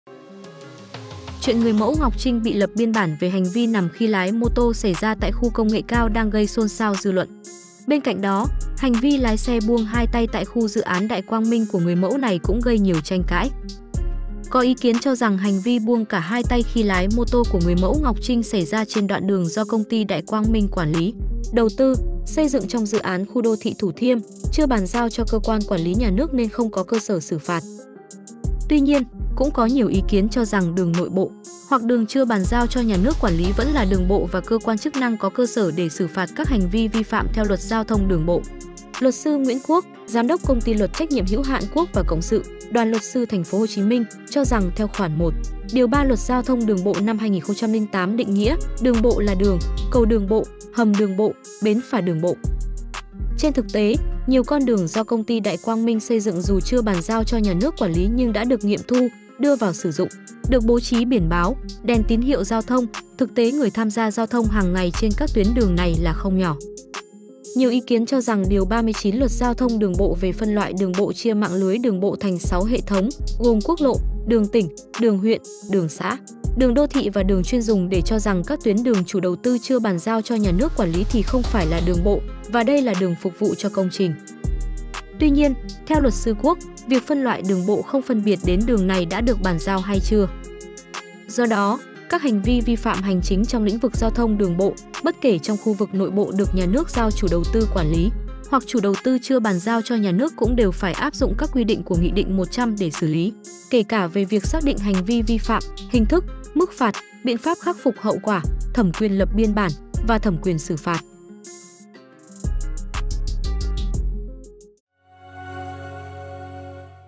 Vụ Ngọc Trinh buông 2 tay lái xe: Đường dự án chưa bàn giao xử phạt được không?
Chuyện người mẫu Ngọc Trinh bị lập biên bản về hành vi nằm khi lái mô tô xảy ra tại Khu công nghệ cao đang gây xôn xao dư luận. Bên cạnh đó, hành vi lái xe buông 2 tay tại khu dự án Đại Quang Minh của người mẫu này cũng gây nhiều tranh cãi.